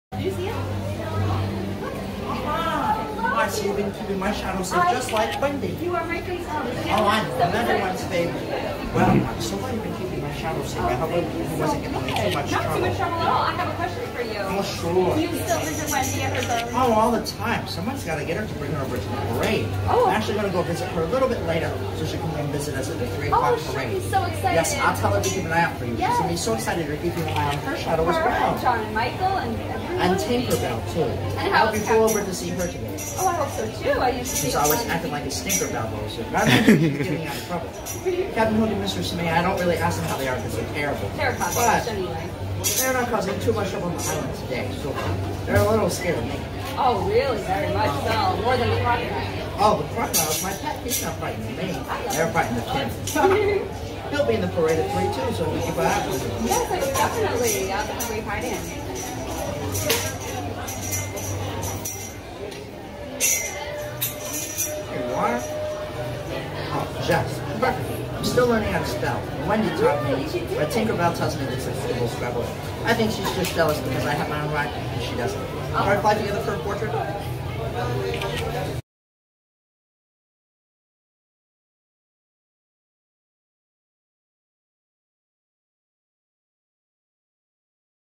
Best Peter Pan Interaction Ever! | Walt Disney World 2020
I have had many character interactions but this one definitely has to be the most memorable! Peter Pan is my favorite and I think he could tell lol! Thank you Peter Pan for spending so much time with me and writing the most amazing message in my autograph book. More Disney World vlogs to come!!

Have a Magical Day and May The Force Be With You
-ThePrincessandTheSith